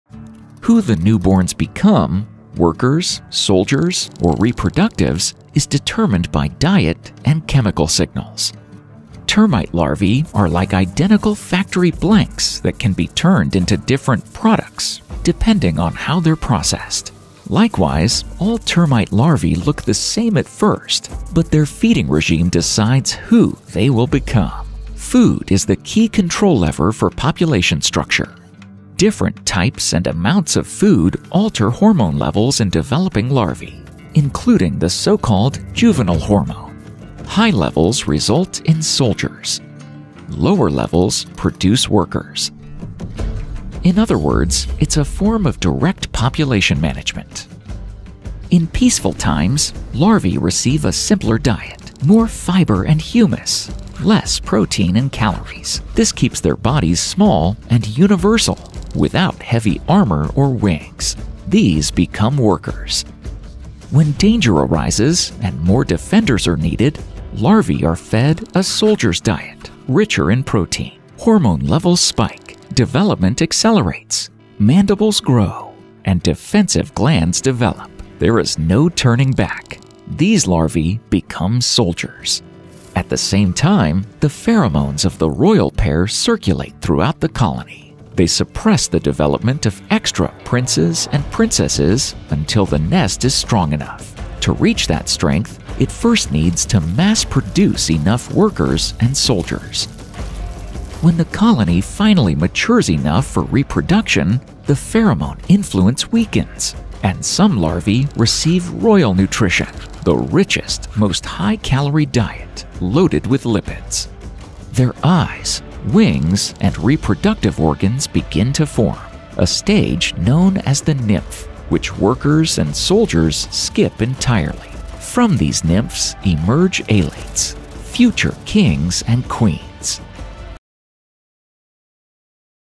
How Termites Decide Who You’ll Become
"The Civilization That Survived the Dinosaurs"
In a termite colony, fate is decided by food.Same larvae — different diets — workers, soldiers, or future kings and queens.